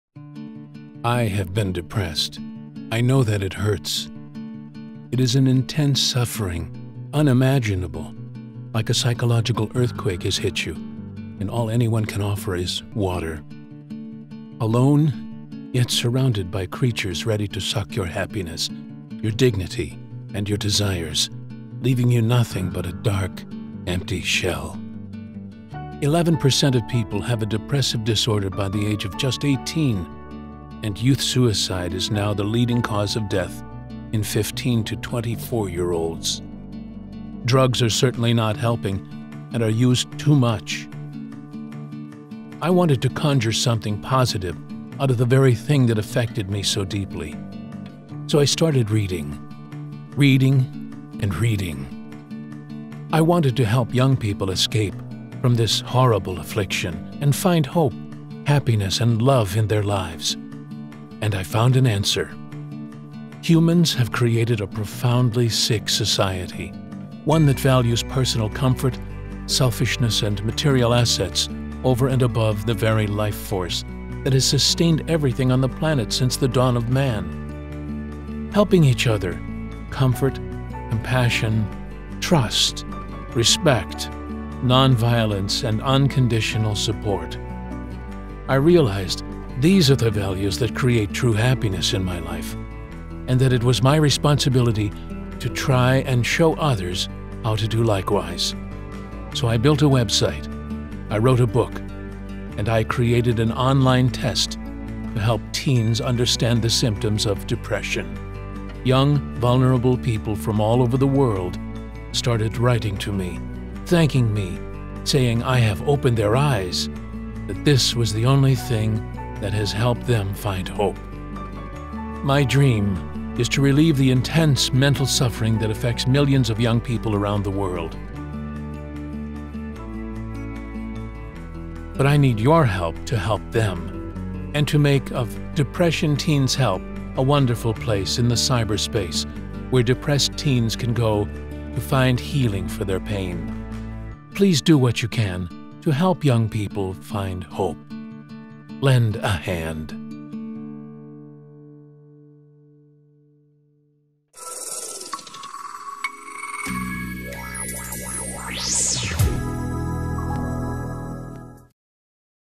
Charity Typography video youth depression
This short inspiring video, with a variety of typefaces and typography, illustrates the message of "how can depressed youth find hope"